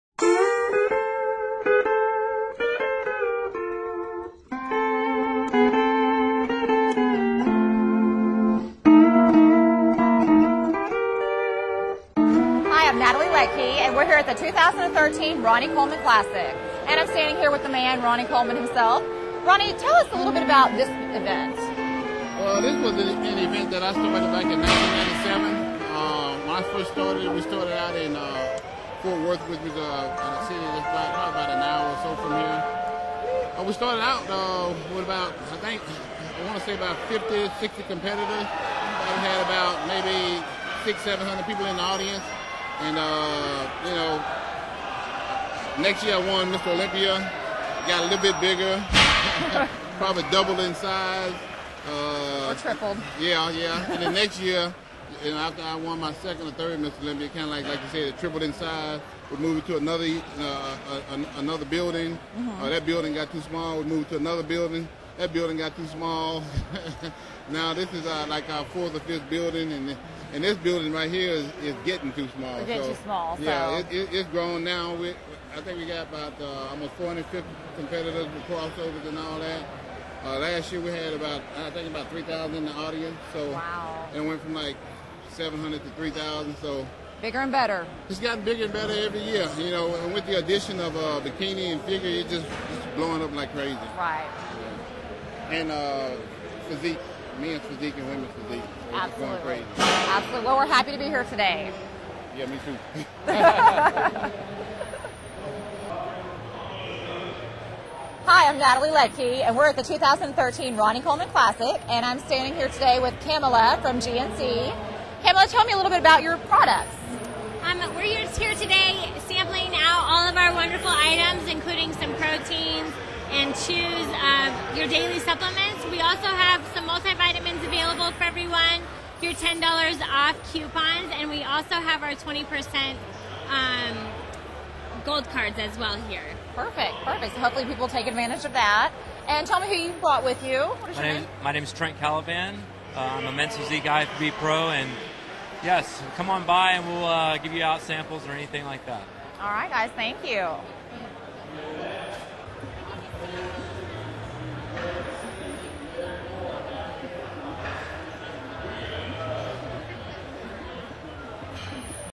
WTFmedia TV "Cool Dallas 3" Ronnie Coleman 2013
Online TV "Cool Dallas".Ronnie Coleman Classic 2013, Talk Radio, Music, and Streaming Videos. Great photography guest and topics.